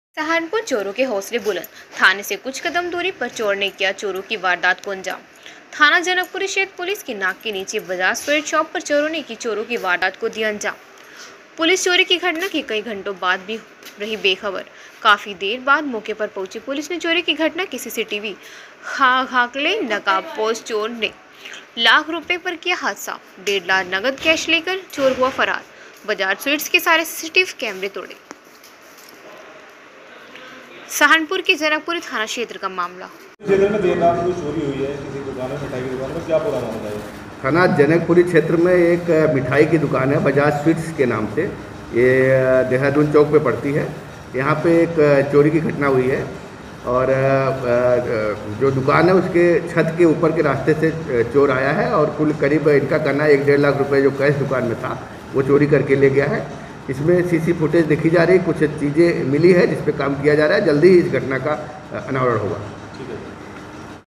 सहारनपुर चोरो के हौसले बुलन्द थाने से कुछ कदम दुरी पर चोर ने दिया चोरी की वारदात को अंजाम ....
सहारनपुर:--थाना जनकपुरी क्षेत्र पुलिस की नाक के नीचे बजाज स्वीट्स शॉप पर चोरों ने चोरी की वारदात को दिया अंजाम

पुलिस चोरी की घटना के कई घंटों बाद तक भी रही बेखबर

काफी देर बाद मौके पर पहुंची पुलिस ने चोरी की घटना के सीसीटीवी खांघाले नकाबपोश चोर ने लाखों रुपए पर किया हाथ साफ

डेढ़ लाख नगद लेकर चोर हुआ फरार बजाज स्वीट्स के सारे सीसीटीवी कैमरे तोड़े

जनपद सहारनपुर के जनकपुरी थाना क्षेत्र का मामला ll

बाइट:--राजेश कुमार
  (एस.पी.सिटी)

अमित मेहरा
ब्यूरो-सहारनपुर